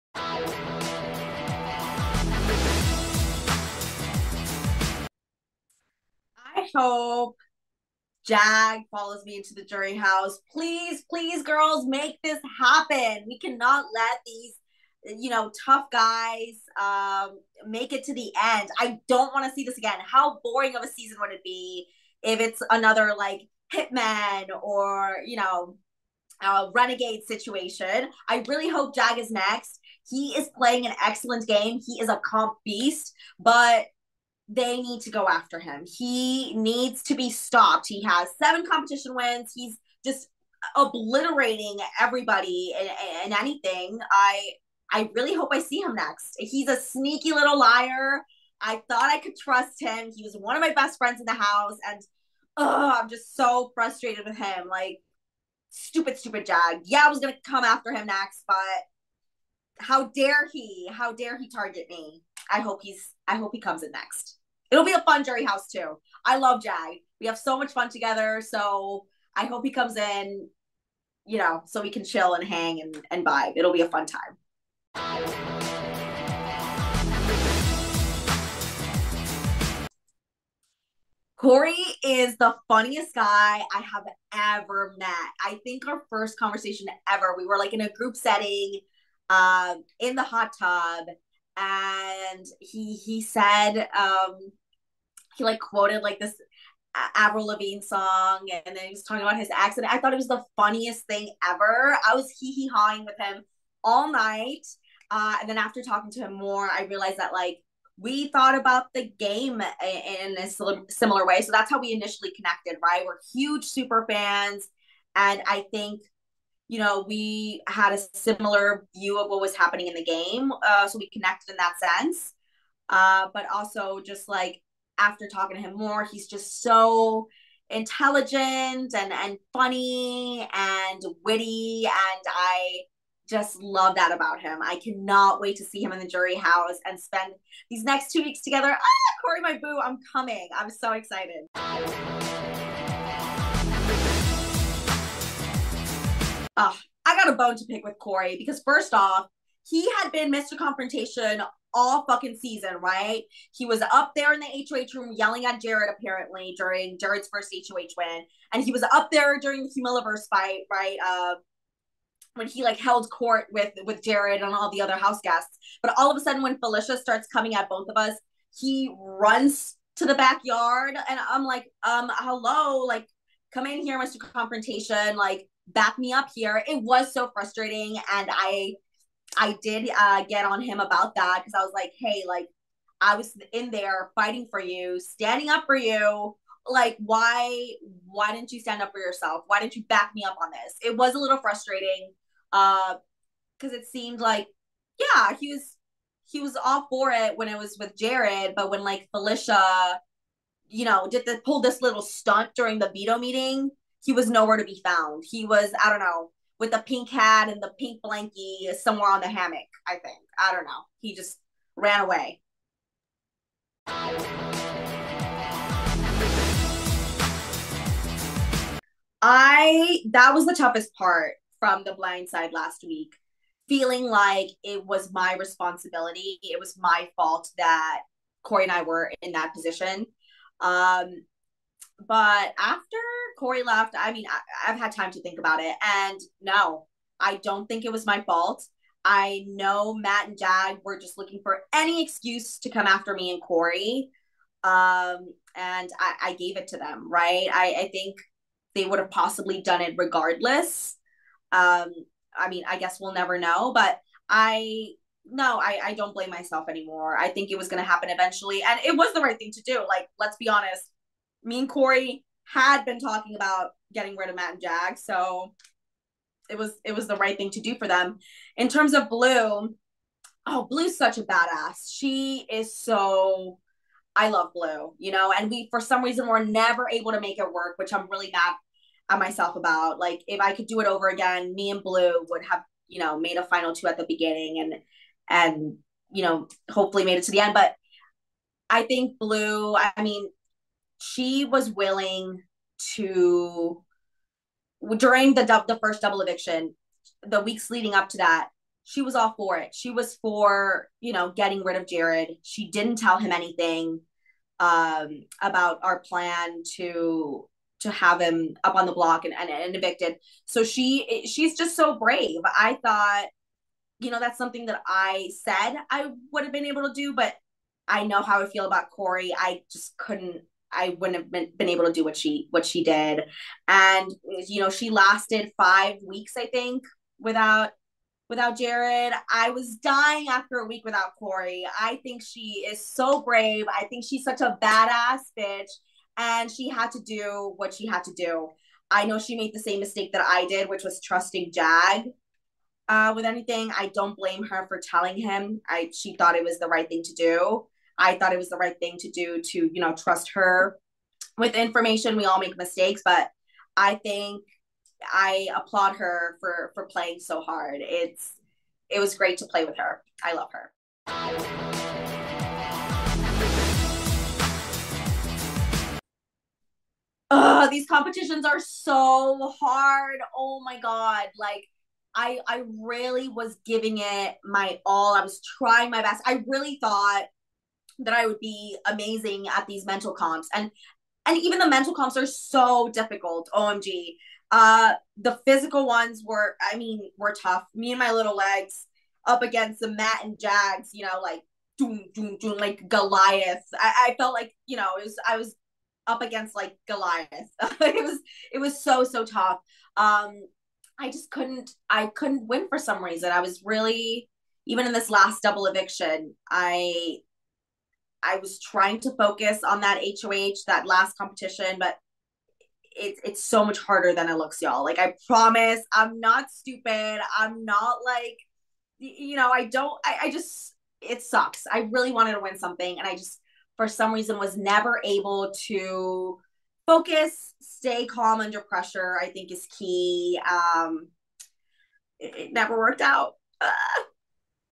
Big Brother 25 - America Lopez Exit Interview
Special Note: The format has changed because we have reached the sensitive jury phase of the game so all of my questions are submitted to CBS, vetted and asked by CBS.

For all your Big Brother 25 news and spoilers go